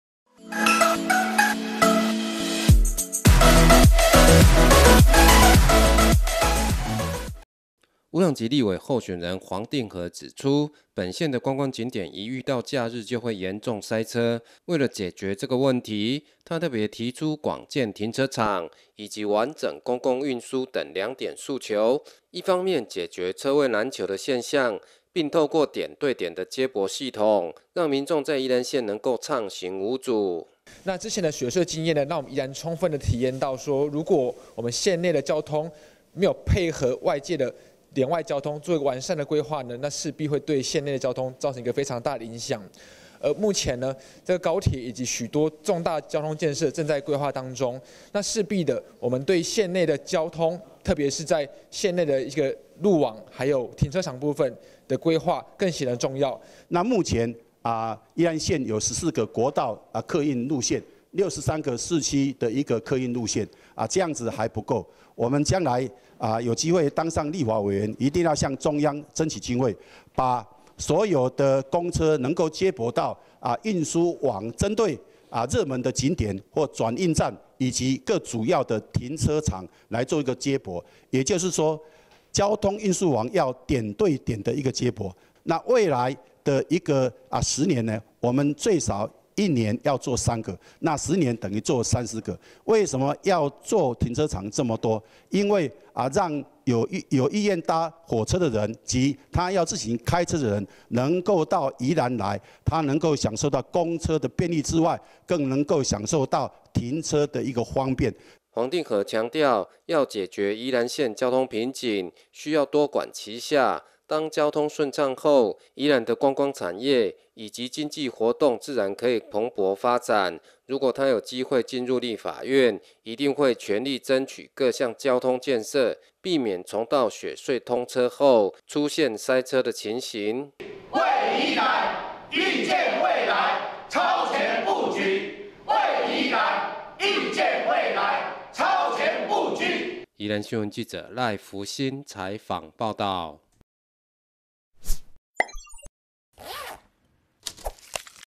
解決塞車 黃定和 建停車場加完整公共運輸
為解決縣內觀光景點每逢假日便塞車現象，黃定和今天特別提出廣建停車場、完整公共運輸等建議，搭配日前所提出的四縱六橫交通路網計畫，藉此打通宜蘭縣整體內外交通的奇經八脈，讓民眾在宜蘭縣交通四通八達暢行無阻